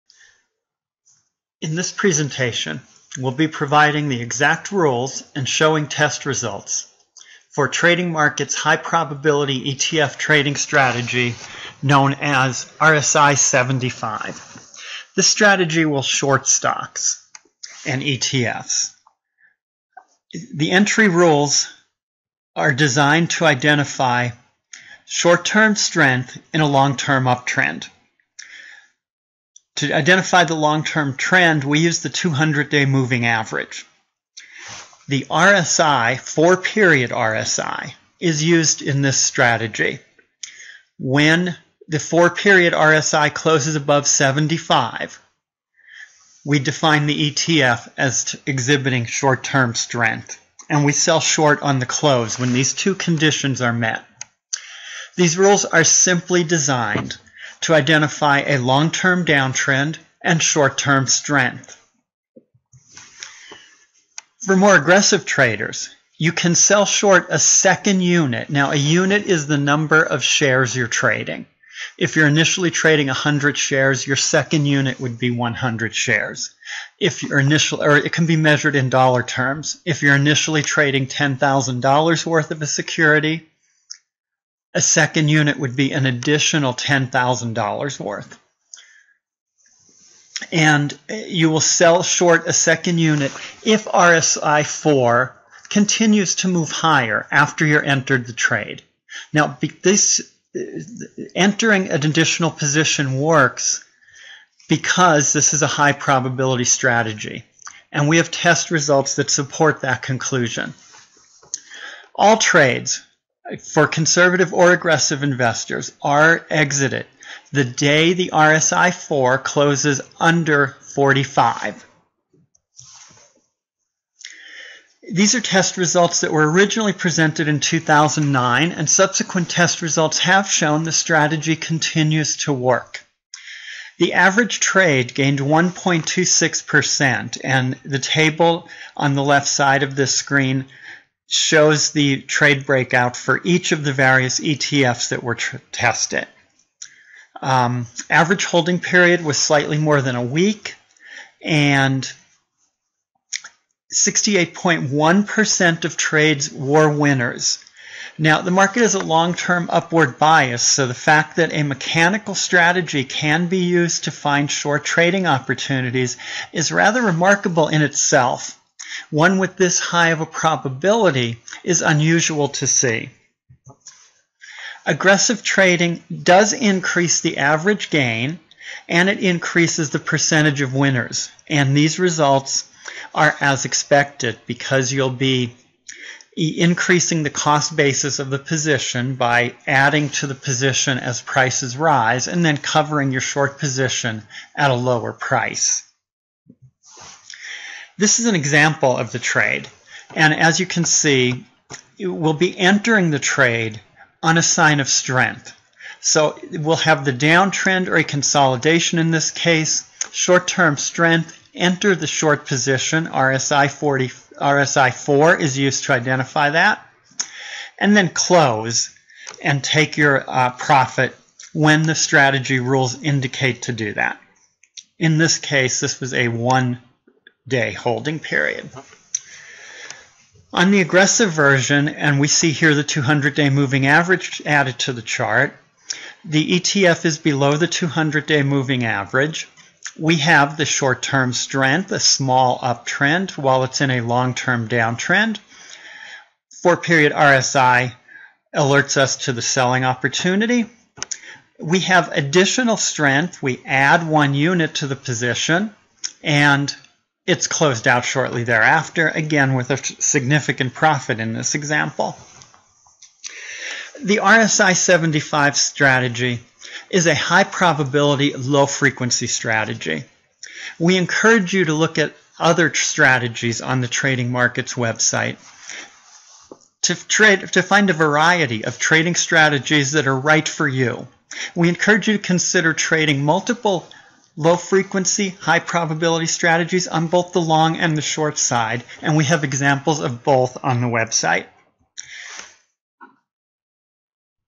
Trading Markets' RSI 75 Trading Strategy
This video shows the rules and results for a simple, high-probability trading strategy.